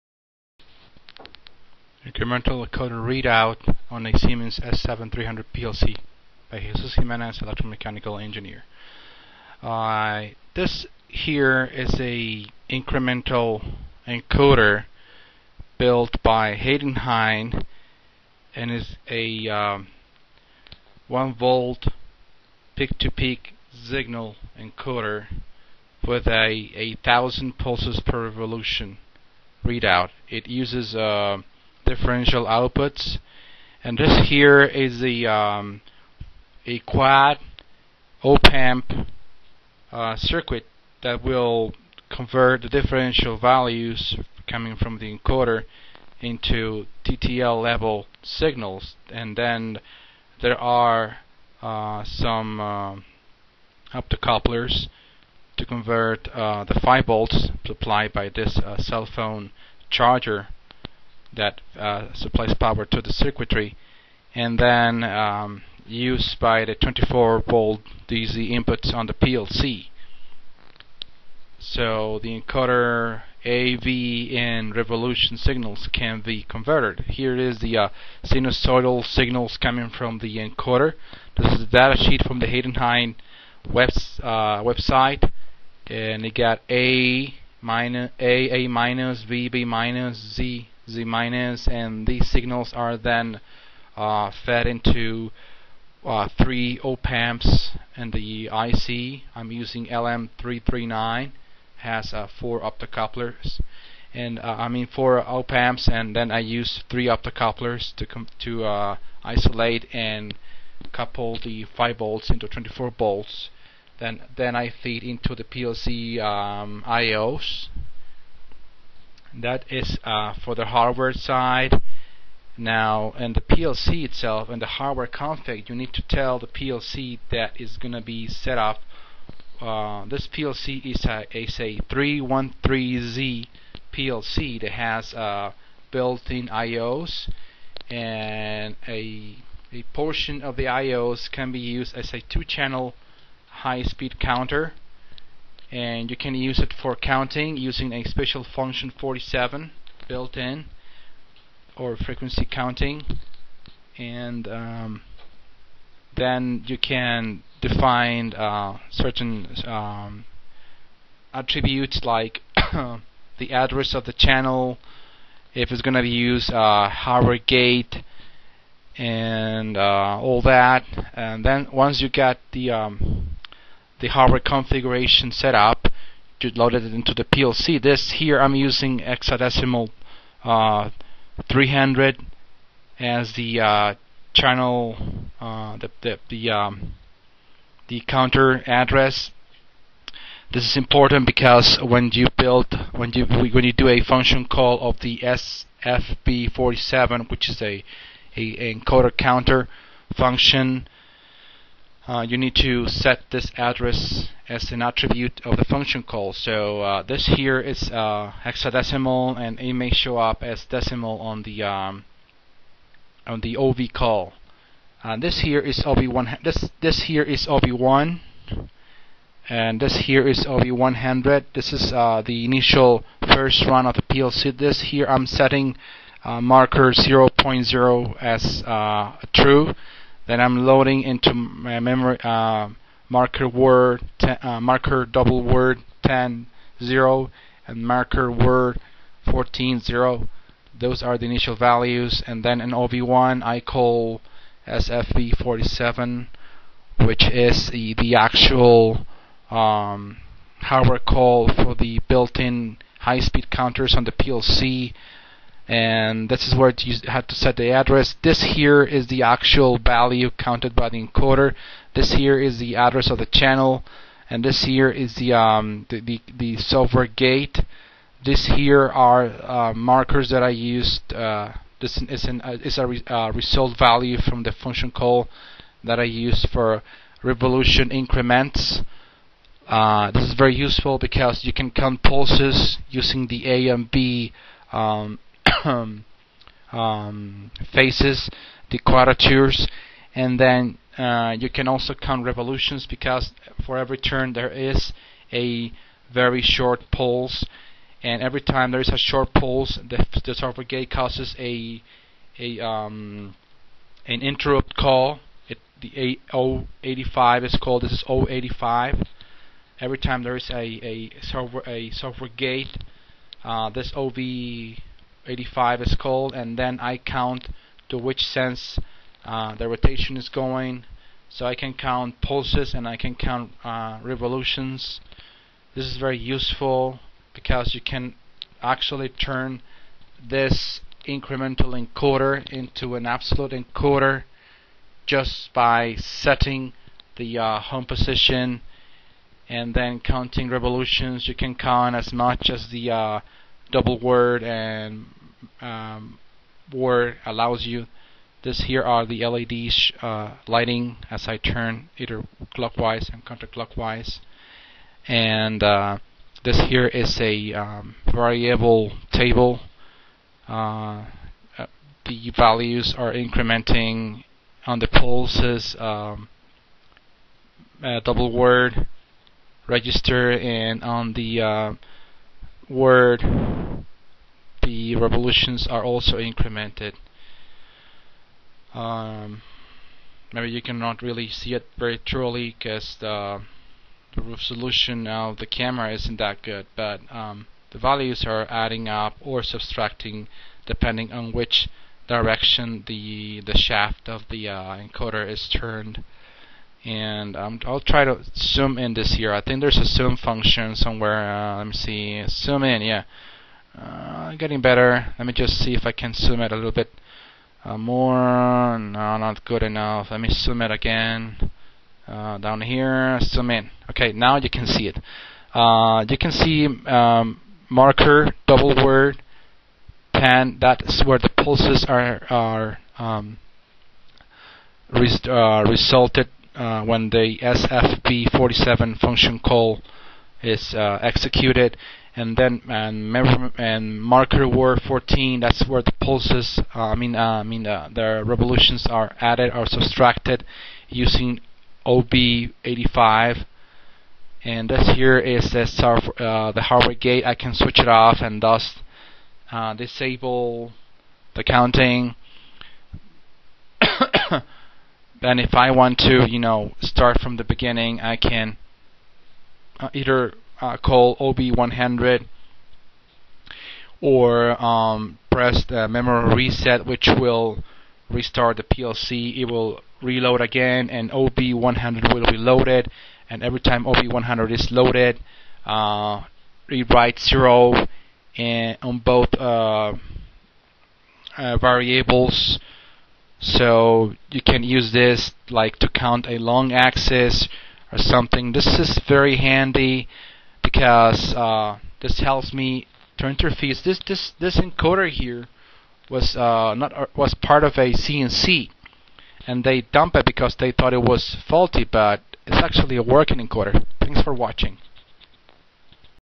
Heidenhain incremental encoder and S7-300 plc
Here I show how I assembled a system to read a rotary incremental encoder and read the shaft angular position on a Siemens S7-300